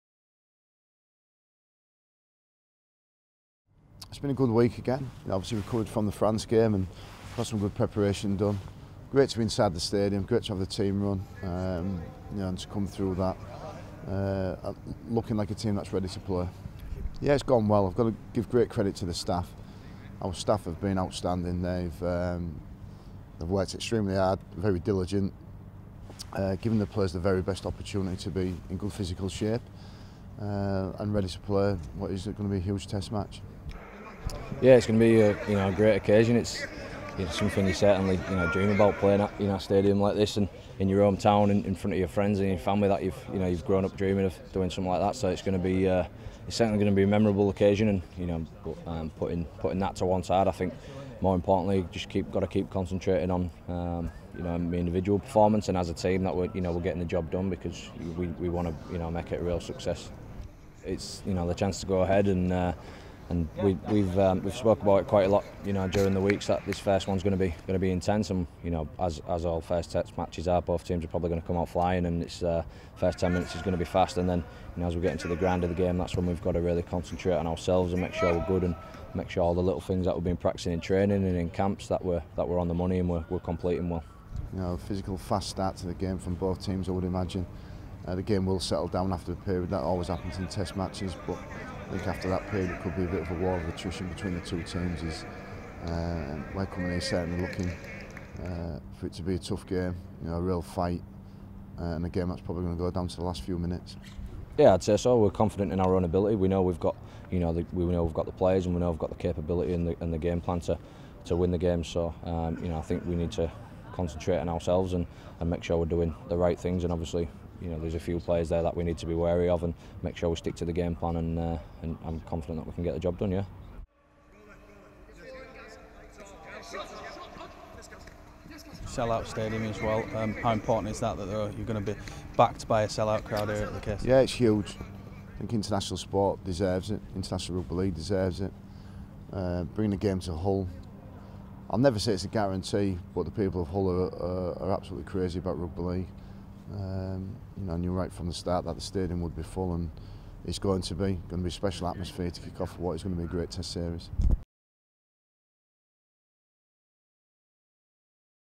Hodgson ready for 'great occasion'
WATCH ‪#‎EnglandRL‬'s final training session at the KC Stadium, plus hear from Hull-born duo Steve McNamara and hooker Josh Hodgson who grew up on the same street in Bilton.